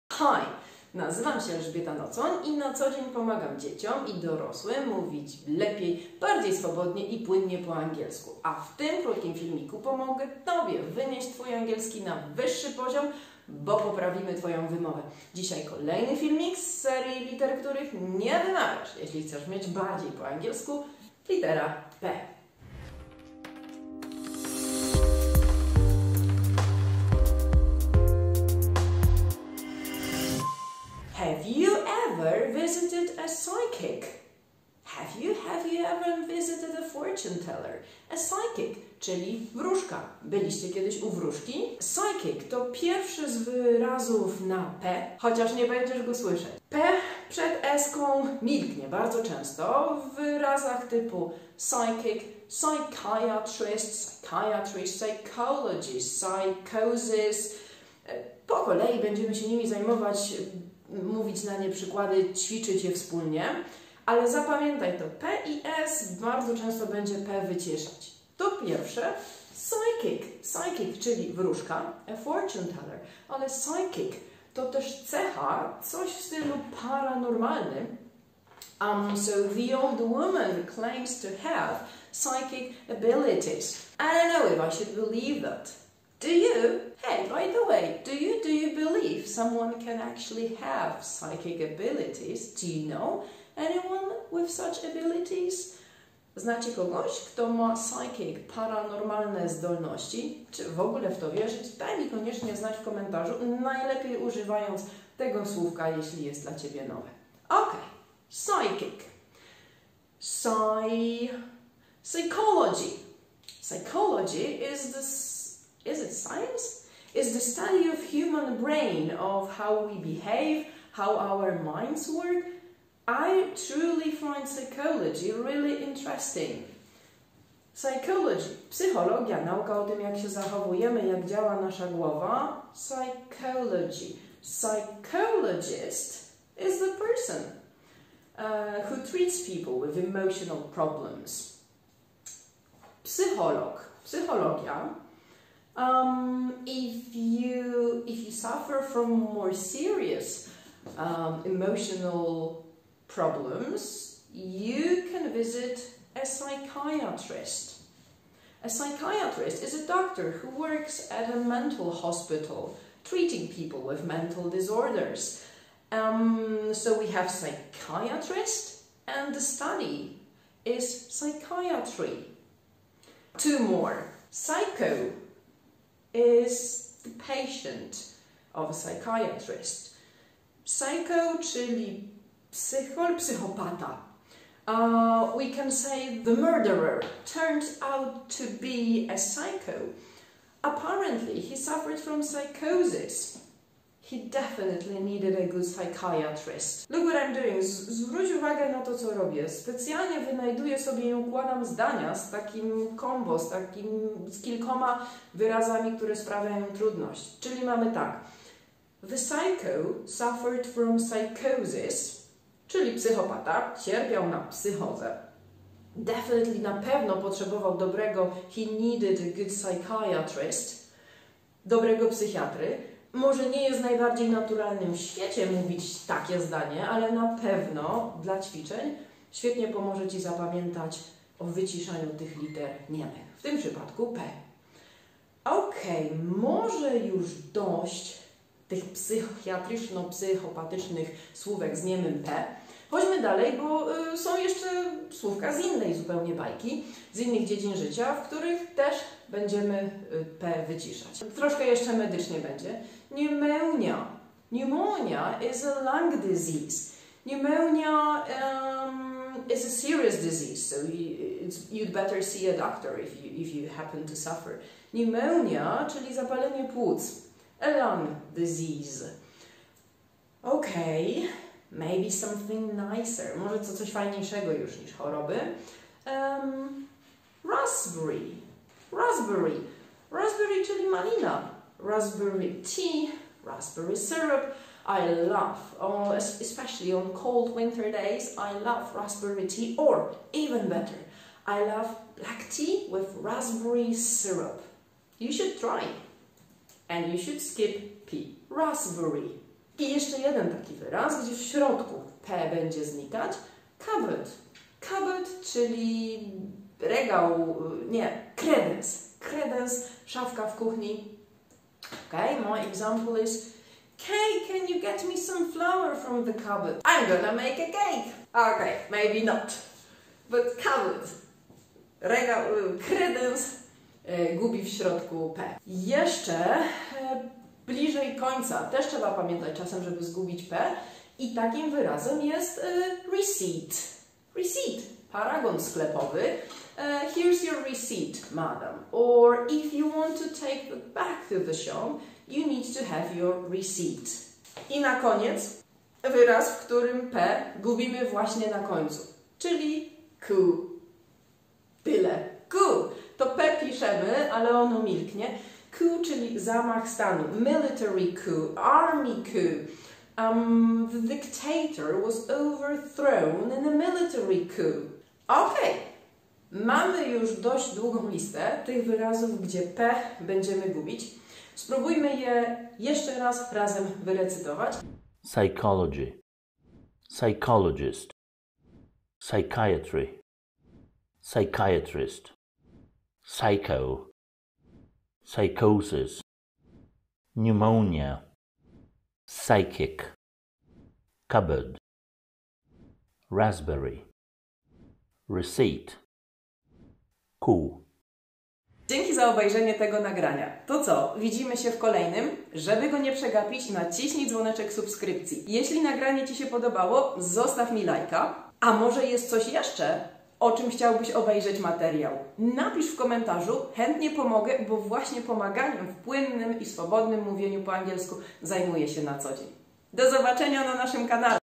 Niema Litera P - wymowa angielska bez tajemnic
Chcesz mówić bardziej po angielsku? Wiedzieć których liter i kiedy nie wymawiać? Dziś nieme P. Bo czy wiesz, że w takich słowach jak „psychic”, „ raspberry” czy „cupboard” litery P się nie wymawia? Dzisiaj wyćwiczysz i opanujesz takie przydatne słowa jak "receipt" czy "psychologist", dzięki temu Twoja wymowa wskoczy na wyższy poziom.

Daj mi znać ile słówek wymawiasz dobrze albo które Cię zaskoczyły!
W filmiku pokazałam tylko kilka słów, jeśli masz niedosyt lub Ci się podobało, pisz śmiało, a nakręcimy więcej filmików!

Do zobaczenia na angielskim w Bochni lub na kursie angielskiego on-line   :-)